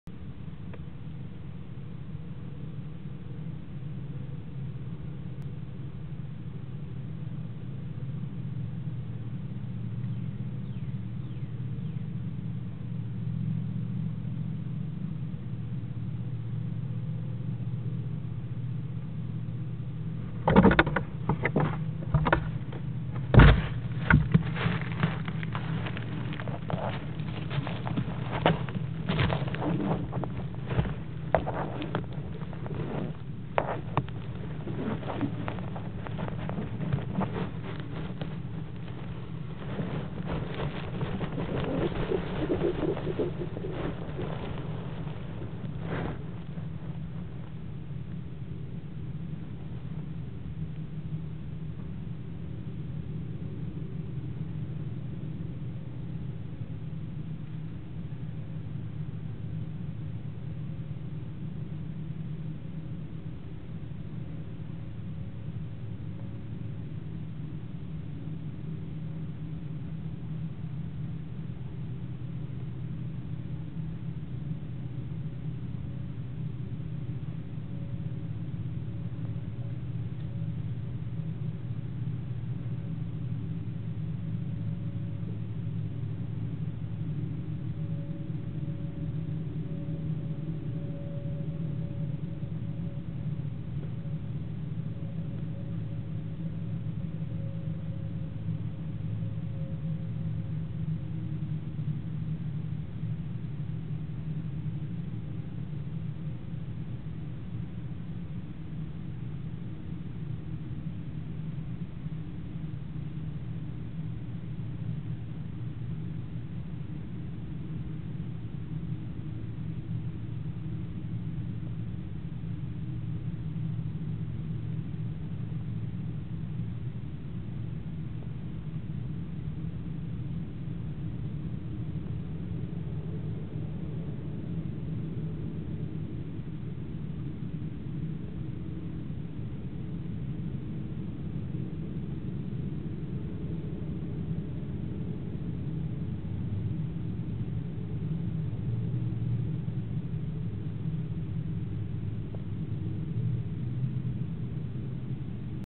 Jan11 2013 06:42 Mrs Barred Owl returns to nest box
Nesting Barred owls in Florida,